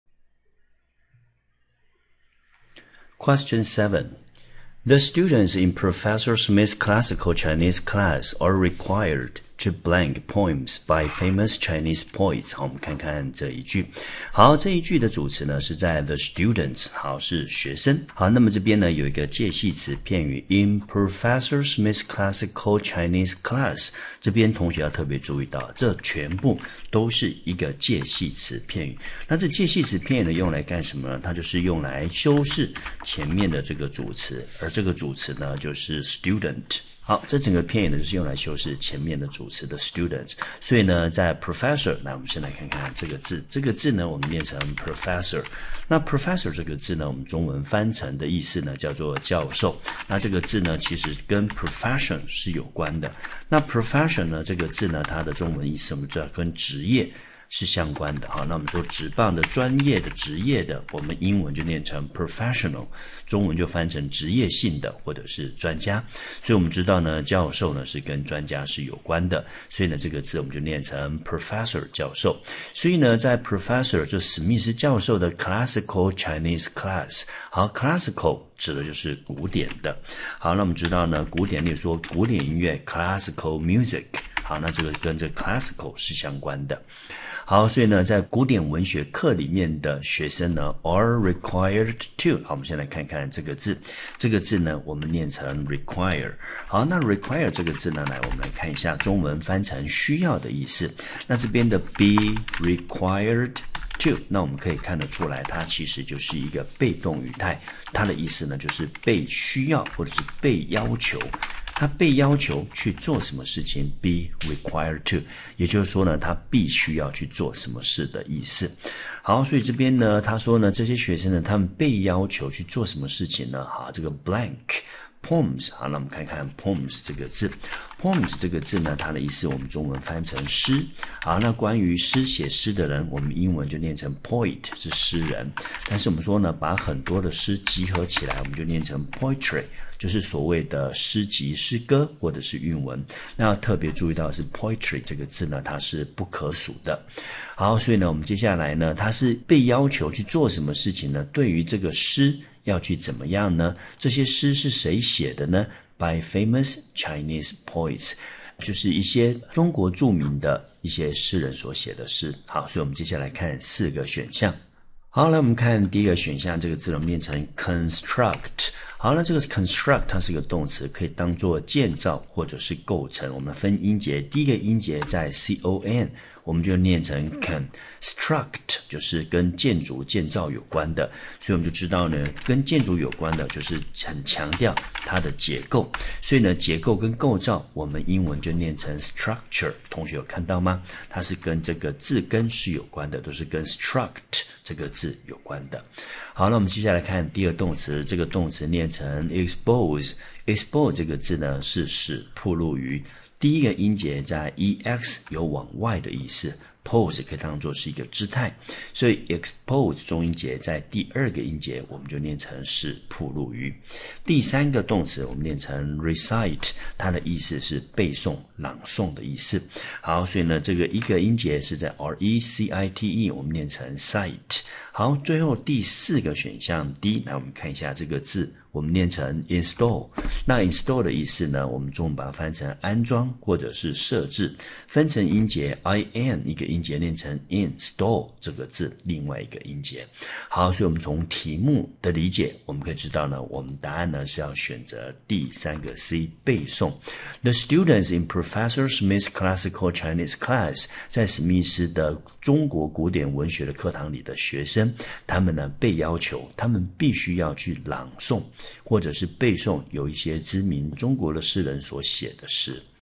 109 大學學測英文科試題第一大題文意選擇：第 7 題
construct (v.) 建造，構成 (n.) 構想；概念
structure (v.) 構造；組織；建造 (n.) 結構；構造；建築物
expose (v.) 使暴露於
recite (v.) 背誦；朗誦；詳述
install (v.) 安裝，設置